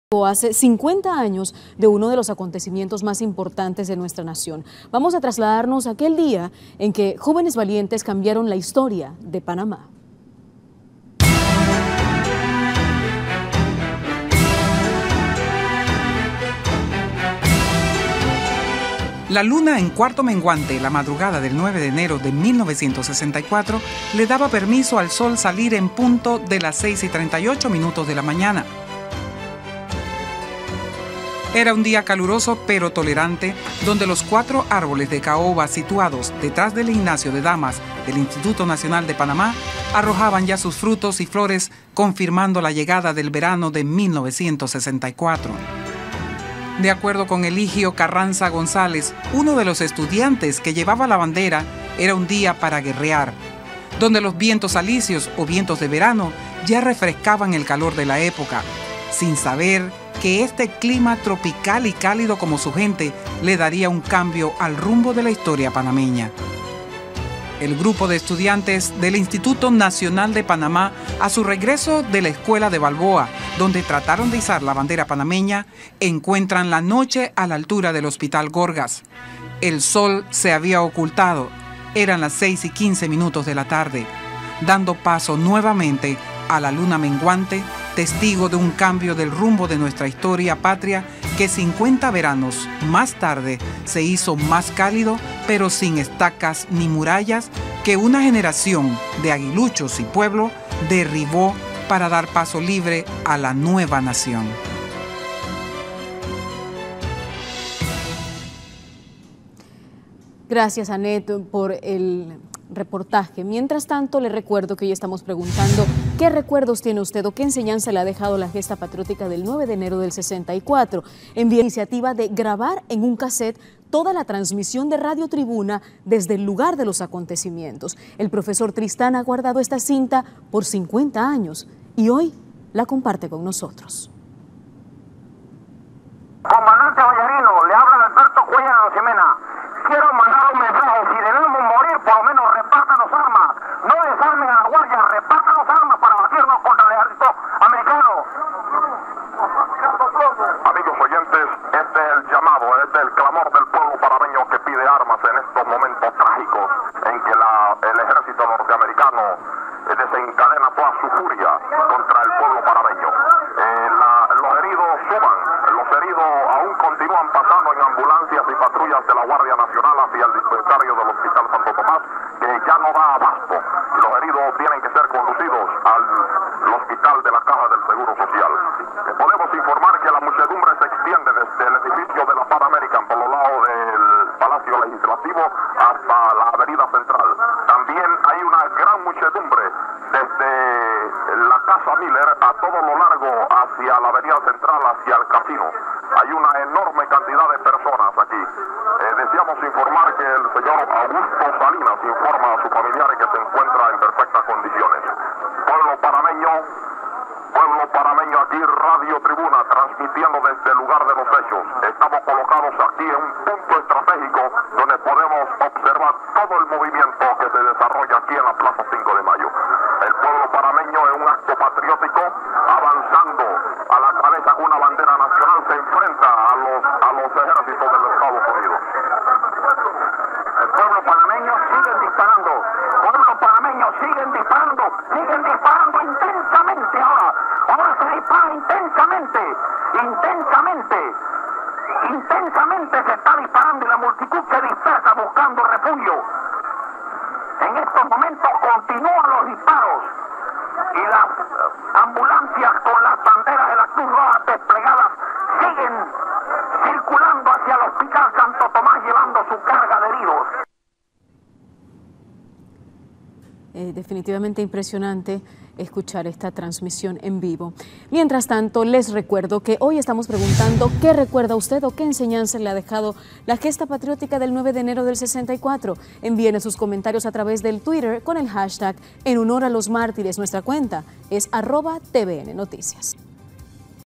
9 de ENERO 1964 / Grabación Histórica de Radio Tribuna
Presentadora: Gisela Tuñón
Voz del reportaje: Annette Quinn
Narración en Vivo: Homero Velásquez y Eduardo Enrique Lim Yueng Quijano.